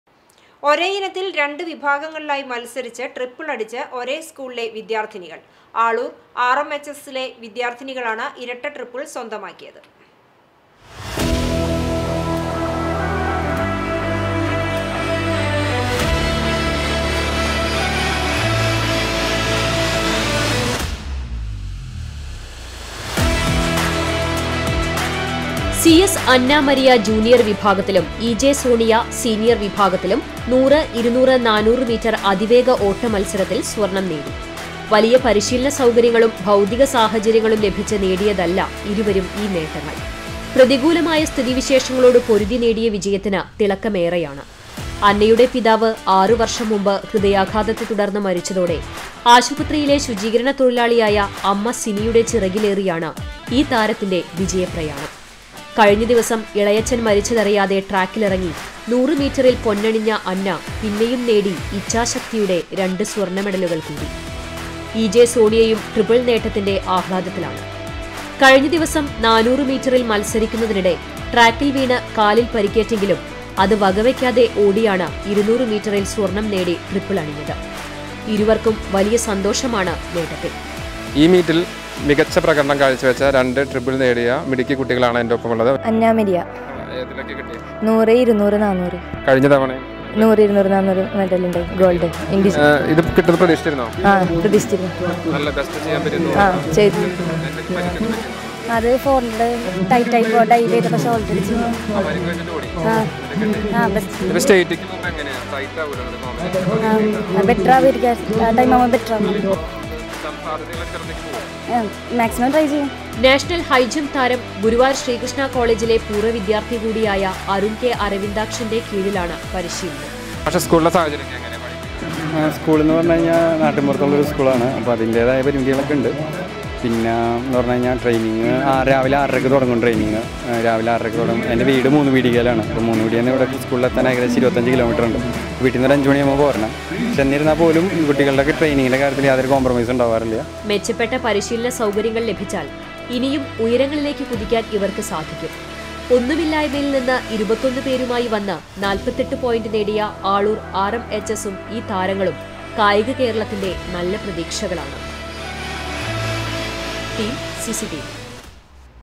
ഡബിളല്ല..ഡബിള്‍ ട്രിപ്പിള്‍
ഒരേ ഇനത്തില്‍ രണ്ട് വിഭാഗങ്ങളിലായി മത്സരിച്ച് ട്രിപ്പിളടിച്ച് ഒരേ സ്‌കൂളിലെ വിദ്യാര്‍ത്ഥിനികള്‍...ആളൂര്‍ ആര്‍.എം.എച്ച്.എസ്.സിലെ വിദ്യാര്‍ത്ഥിനികളാണ് ഇരട്ട ട്രിപ്പിള്‍ സ്വന്തമാക്കിയത്.